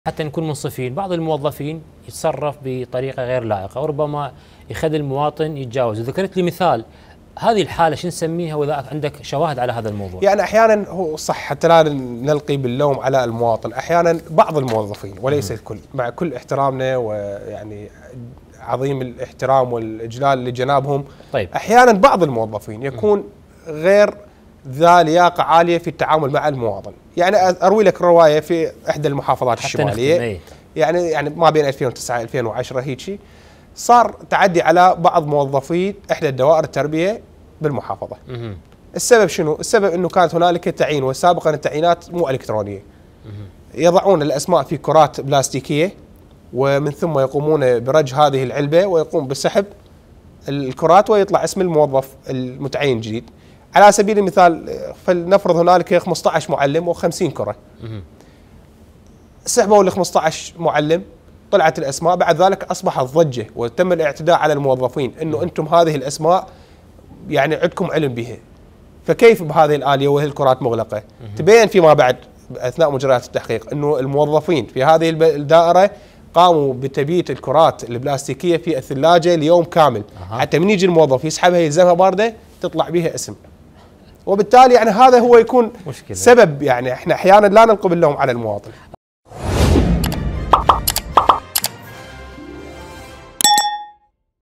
تلاعب بتوظيف معلمي صلاح الدين
يبيتون كرة القرعة بالثلاجة ومن يجي الموظف يعرفها باردة ويسحبها.. مختص في الشأن القانوني يروي قصة التلاعب بقرعة توظيف المعلمين في صلاح الدين